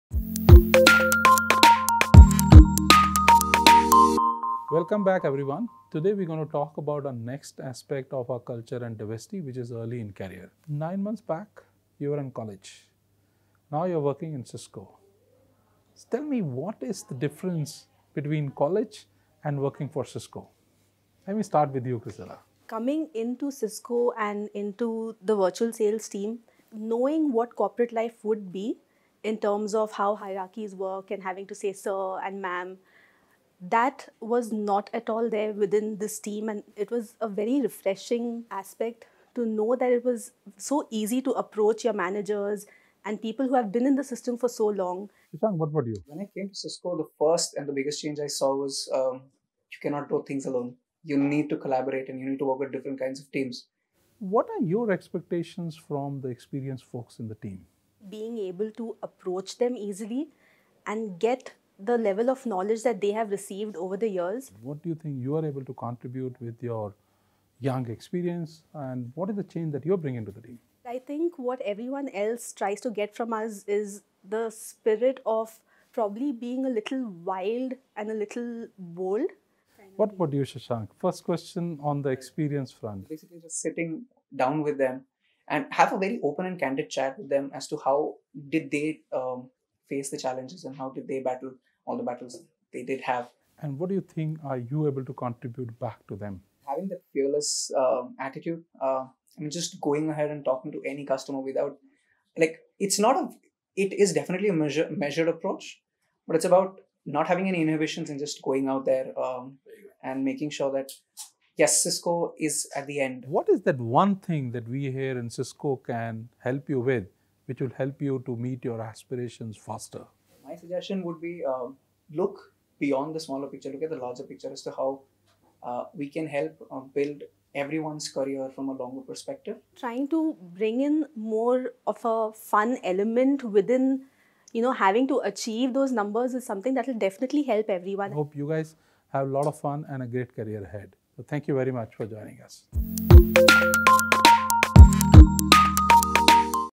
Diversity Dialogues: Early in Career
DiversityDialogues: Early in Career- Criselda Pinto and Sashank Krishnan talk about how Cisco encourages young talent and helps them achieve their career goals.